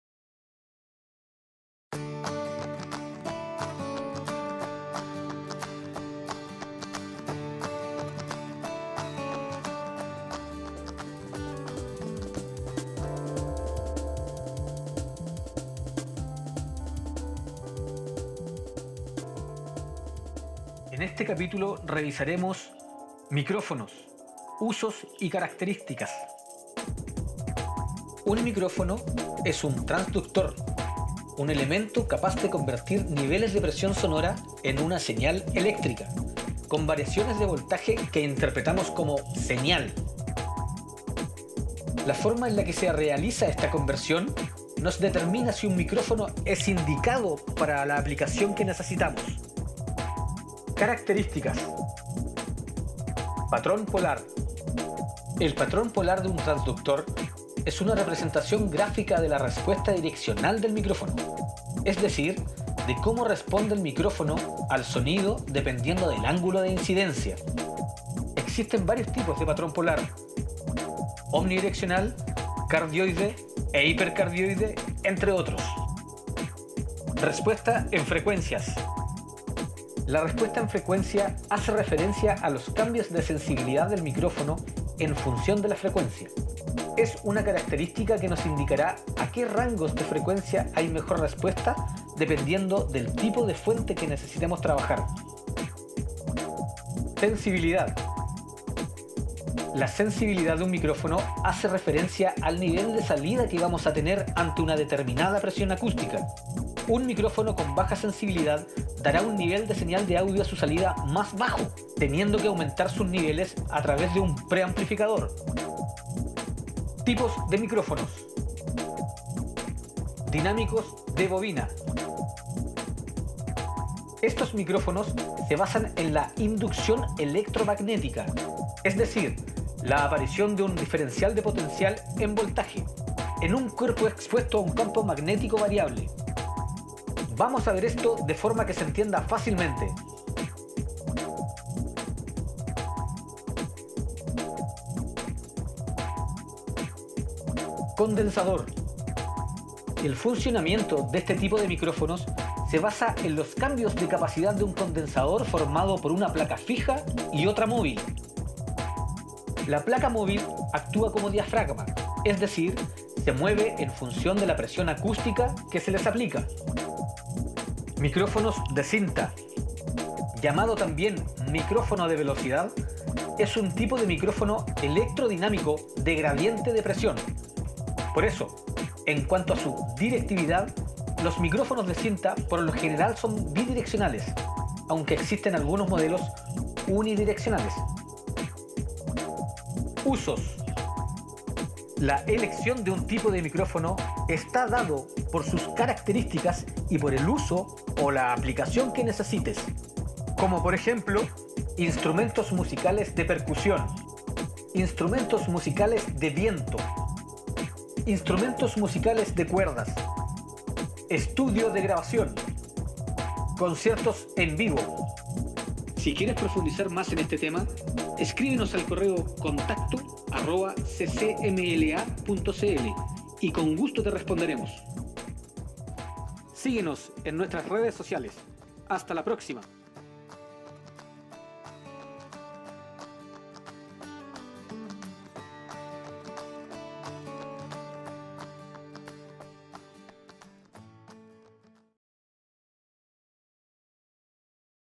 Micrófonos, usos y características.
En esta tercera cápsula, las y los invitamos a conocer más sobre los usos y características del micrófono.